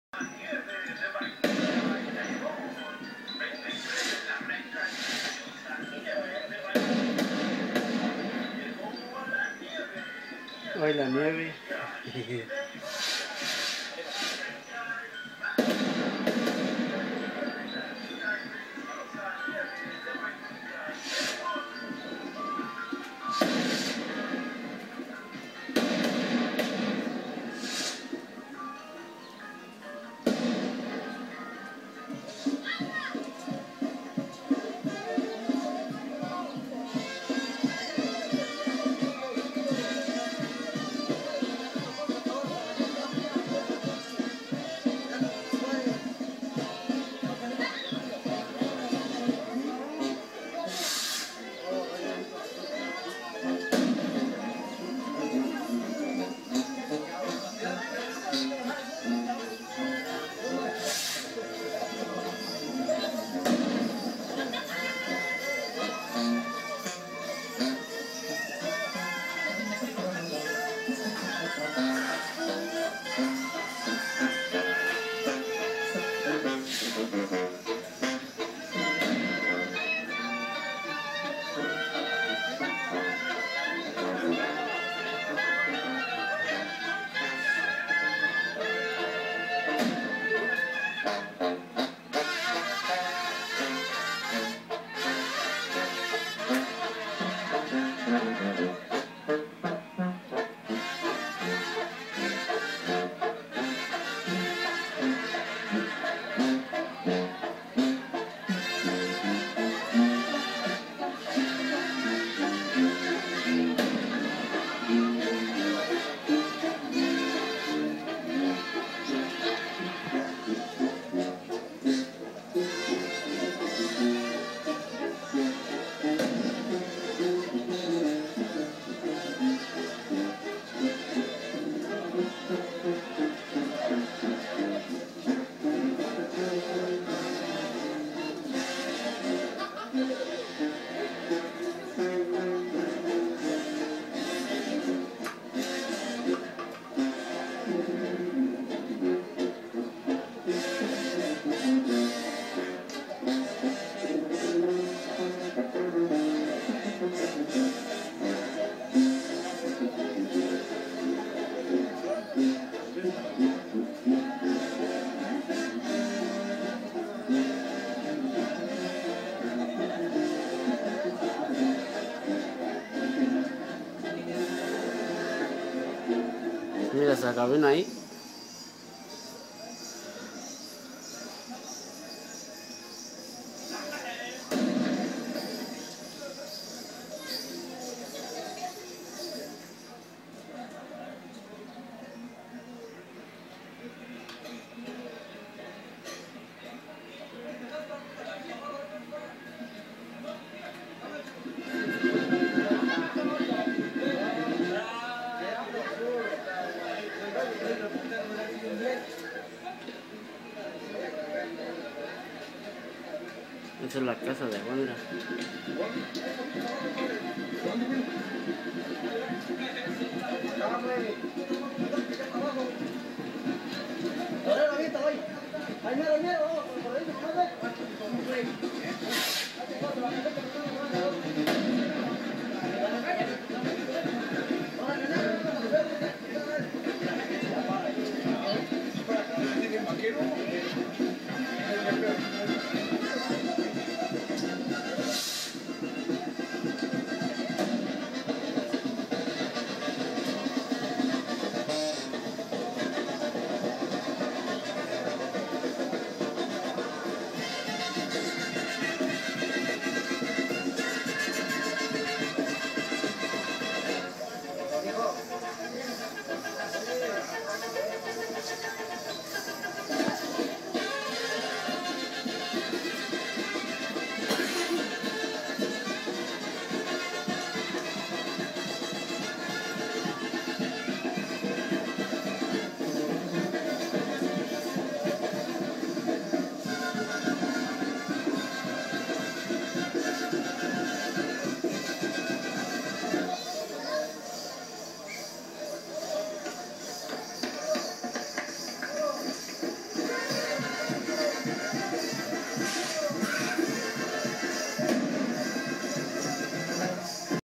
Fiestas de mayo 2010 #3
Pasiando nuestro señor San isidro por todo el rancho